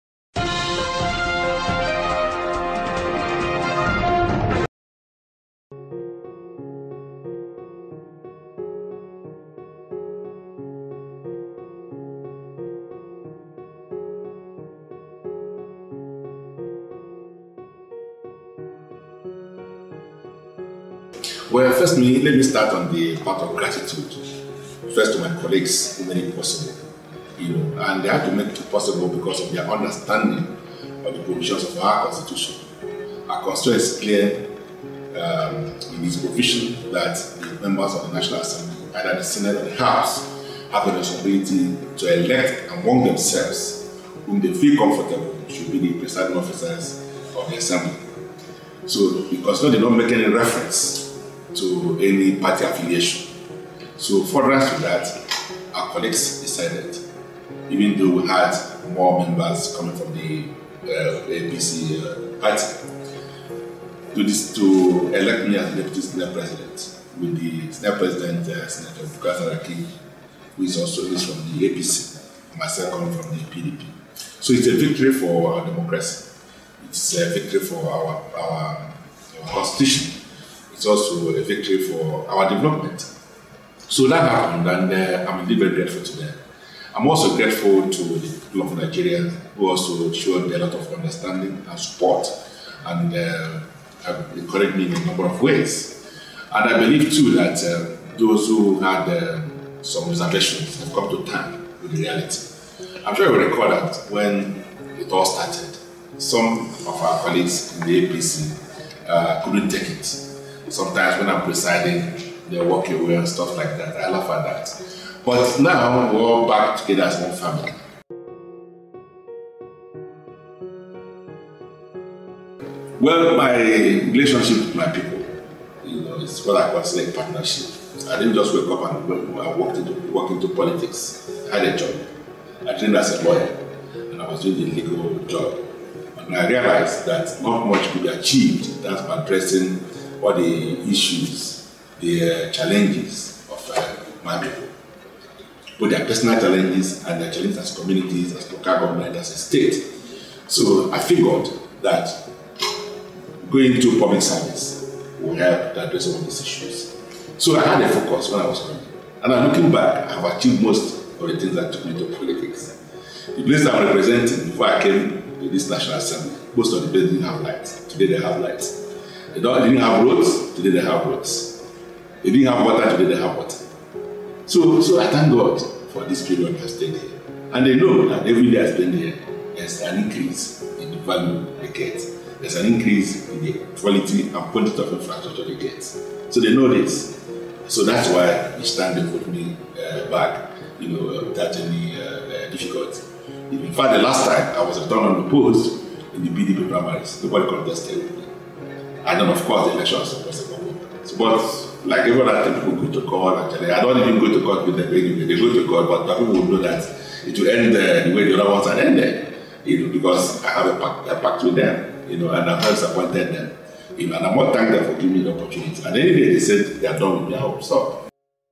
Ekweremadu : How I became deputy senate president
Deputy Senate President Ike Ekweremadu speaks on how he became the deputy and his relationship with his constituents (Enugu West).